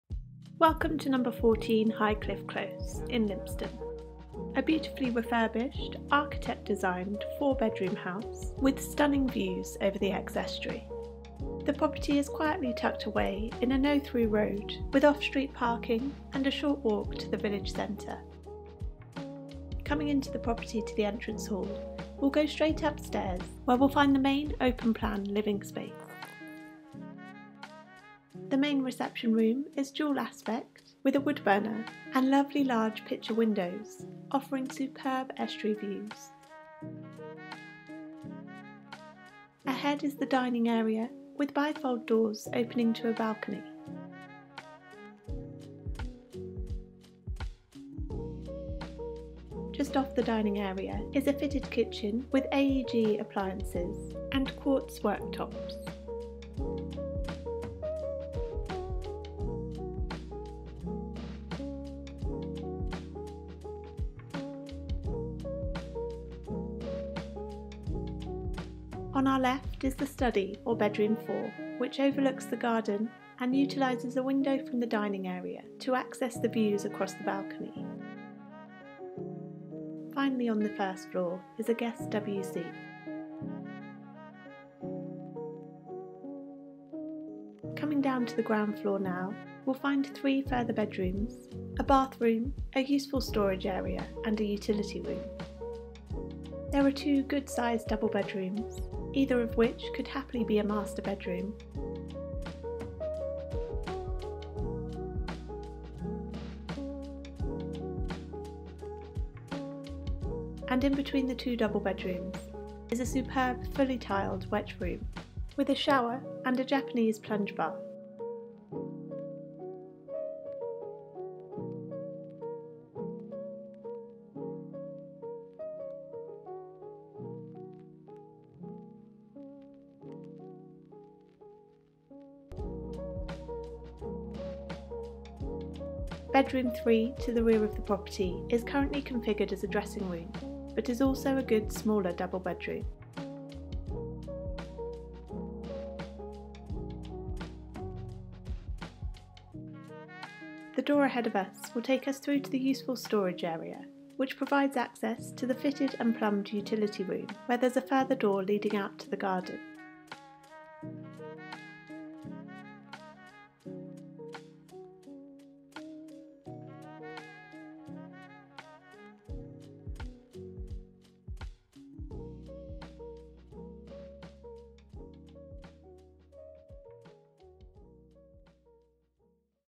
14 Highcliffe Close | Four Bedroom Architect Designed House, a short walk into Lympstone Village
14 Highcliffe Close Video Tour | Four Bedroom Architect Designed House, a short walk into Lympstone Village

A beautifully refurbished Architect designed four bedroom house with stunning views over the Exe Estuary. The property has been designed and extended to capitalise on the wonderful views and maximise light and space with reverse level living. Quietly tucked away in a no through road, with off street parking and a short walk to the village centre.